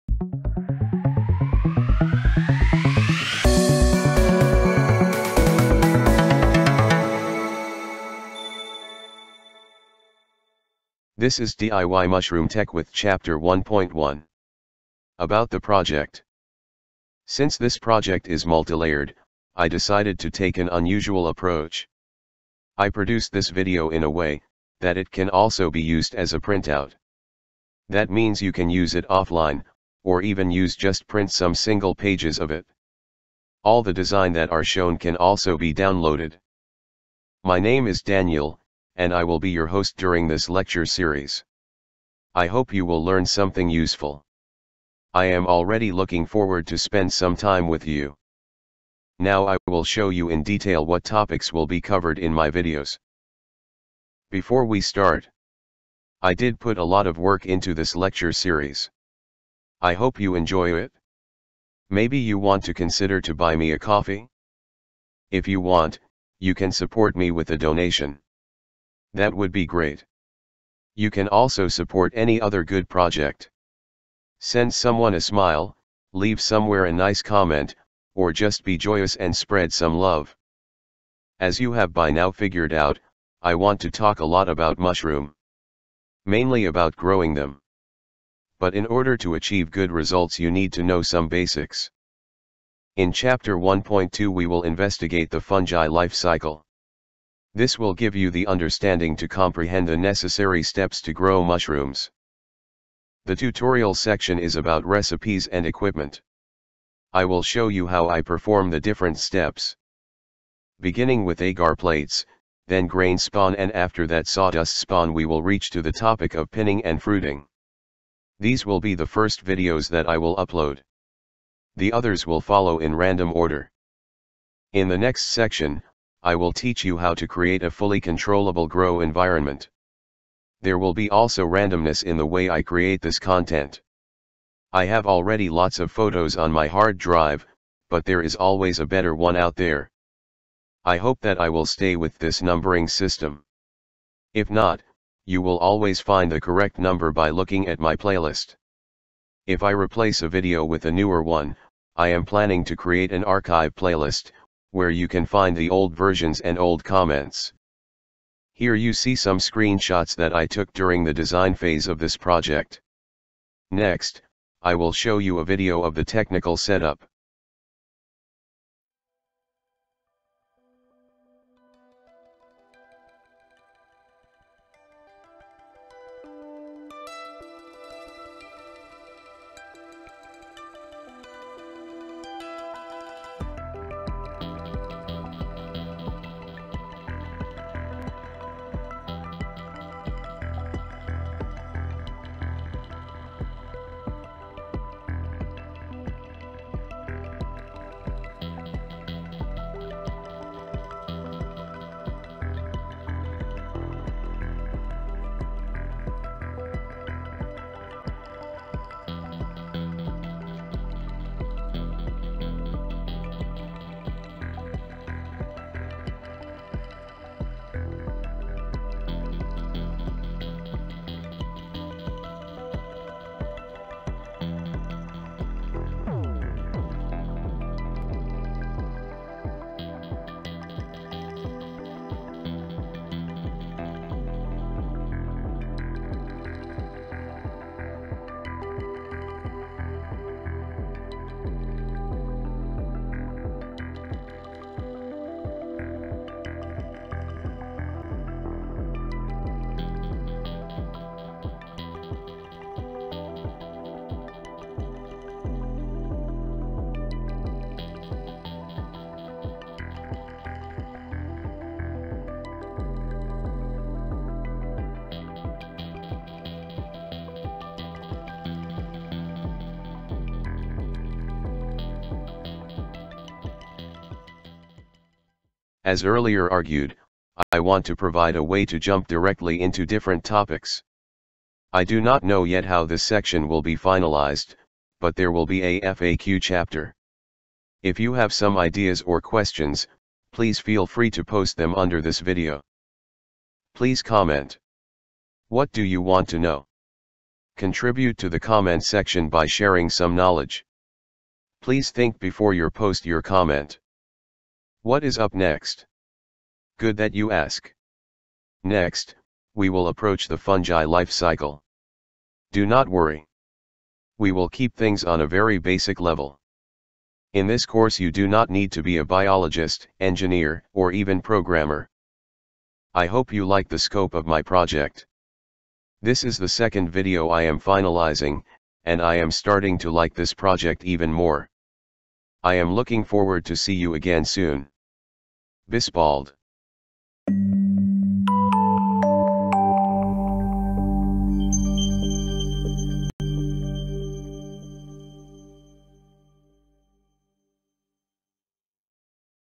1.1 About the Project
⚙️➡️🍄 In this video, I will provide an overview of the topics that will be covered in the tutorial. Cultivating mushrooms can be easy, but there are certain aspects of the process that need to be understood in order to achieve success. I will explain in detail what you need to know to get started with your own mushroom growing operation.

🍄 Here you will find the .pdf script for printing and offline use!

🛠️ Here you will find the .stl files for 3D printing!

🕘✅🍄 Jump marks:
◼ Introduction: 0:00
◼ Support: 0:52
◼ About the project: 1:21
◼ About the tutorials: 1:50
◼ About the TEK with demo: 2:11
◼ FAQs: 5:49
◼ Closure: 5:08